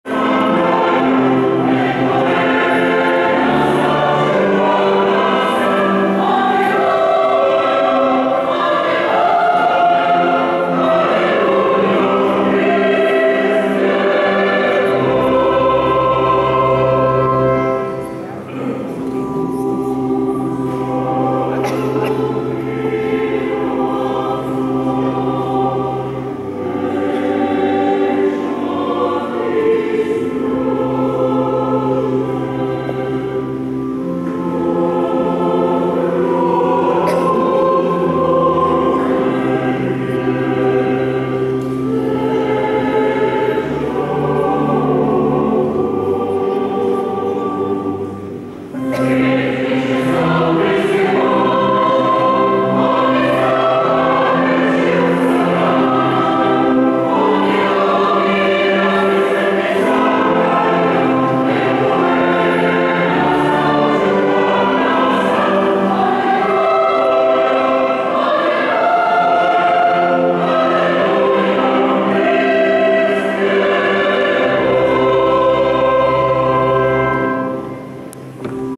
Choir and Easter Communion at the Sacred Heart Cathedral Sarajevo
Easter Sunday Communion Mass at the Sacred Heart (Roman Catholic) Cathedral in Sarajevo, Bosnia & Hercegovina. The choir is amazing!